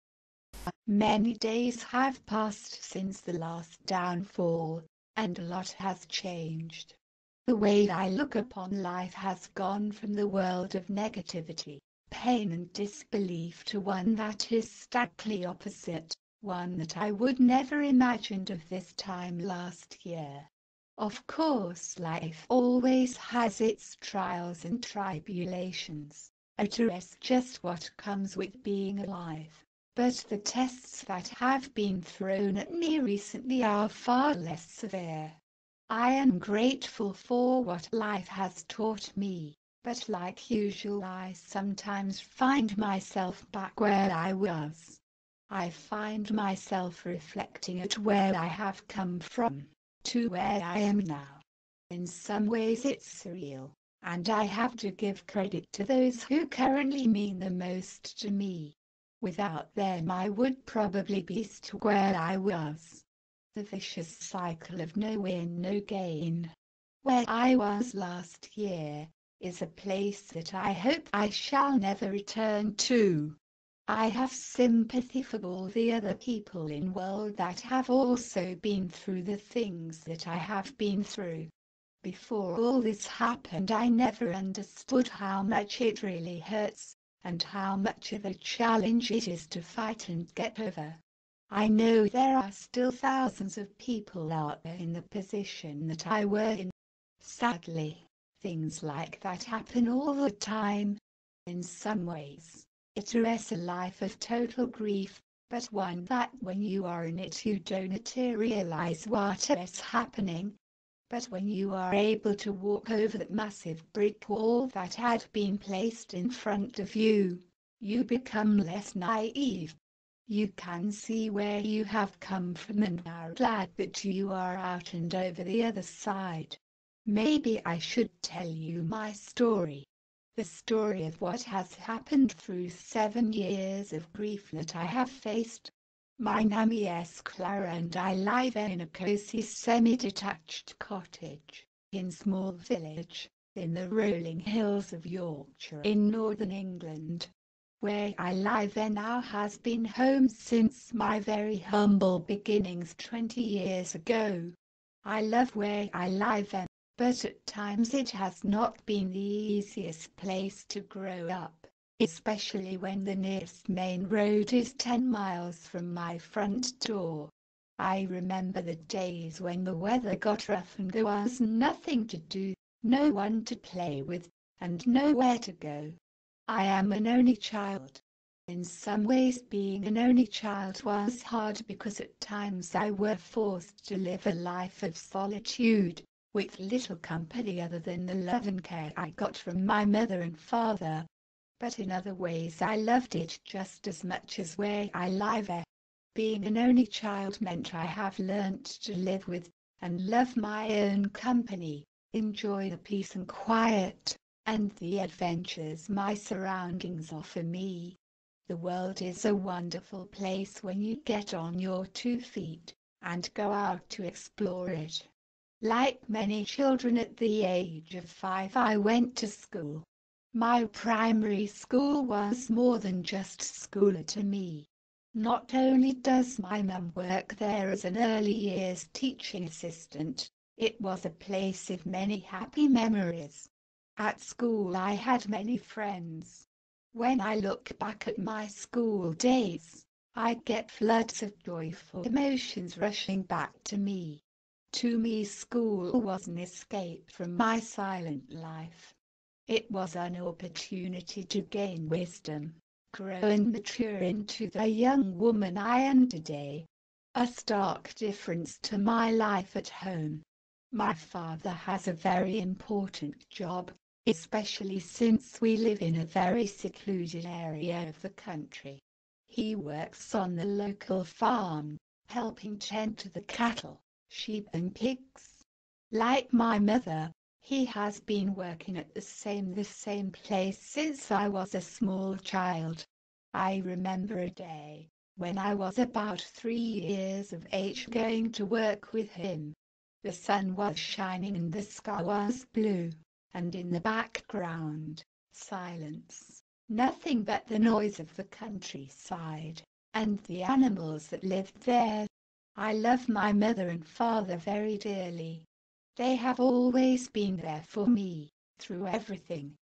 The Trials and Tribulations of Clara Jones (audio Story) Chapter one
(a story of a young girls reflection on demostic violence.)

The Trials and Tribulations of Clara Jones:  Chapter one

Many days have passed since the last downfall, and a lot has changed. The way I look upon life has gone from the world of negativity, pain and disbelief to one that is starkly opposite, one that I would never imagined of this time last year. Of course life always has its trials and tribulations, that's just what comes with being alive, but the tests that have been  thrown at me recently are far less severe. I am grateful for what life has taught me, but like usual I sometimes find myself back where I was. I find myself reflecting at where I have come from, to where I am now. In some ways its surreal, and I have to give credit to those who currently mean the most to me. Without them I would probably be stuck where I was. The vicious cycle of no win no gain.

    Where I was last year, is a place that I hope I shall never return too. I have sympathy for all the other people in world that have also been through the things that I have been through. Before all this happened I never understood how much it really hurts, and how much of a challenge it is to fight and get over. I know there are still thousands of people out there in the position that I were in. Sadly, things like that happen all the time. In some ways, it's a life of total grief, but one that when you are in it you don't realize what's happening. But when you are able to walk over that massive brick wall that had been placed in front of you, you become less naive. You can see where you have come from and are glad that you are out and over the other side.

   Maybe I should tell you my story. The story of what has happened through seven years of grief that I have faced. My name's Clara and I live in a cosy semi detached cottage, in small village, in the rolling hills of Yorkshire in northern England. Where I live now has been home since my very humble beginnings twenty years ago. I love where I live, but at times it has not been the easiest place to grow up, especially when the nearest main road is ten miles from my front door. I remember the days when the weather got rough and there was nothing to do, no one to play with, and no-where to go. I am an only child. In some ways being an only child was hard because at times I were forced to live a life of solitude, with little company other than the love and care I got from my mother and father. But in other ways I loved it just as much as where I live. Being an only child meant I have learnt to live with, and love my own company, enjoy the peace and quiet, and the adventures my surroundings offer me. The world is a wonderful place when you get on your two feet, and go out to explore it.

     Like many children at the age of five I went to school. My primary school was more than just 'school' to me. Not only does my mum work there as an early years teaching assistant, it was a place of many happy memories. At school I had many friends.  When I look back at my school days, I get floods of joyful emotions rushing back to me. To me school was an escape from my silent life. It was an opportunity to gain wisdom, grow and mature into the young woman I am today. A stark difference to my life at home.

     My father has a very important job, especially since we live in a very secluded area of the country. He works on the local farm, helping tend to the cattle, sheep and pigs. Like my mother, he has been working at the same the same place since I was a small child. I remember a day, when I was about 3 years of age going to work with him. The sun was shining and the sky was blue, and in the background, silence, nothing but the noise of the countryside, and the animals that lived there. I love my mother and father very dearly. They have always been there for me, through everything, no matter what. I couldn't wish for better parents. I still live at home, well I should say, I have returned home after I left my ex behind. But this is only where my story begins.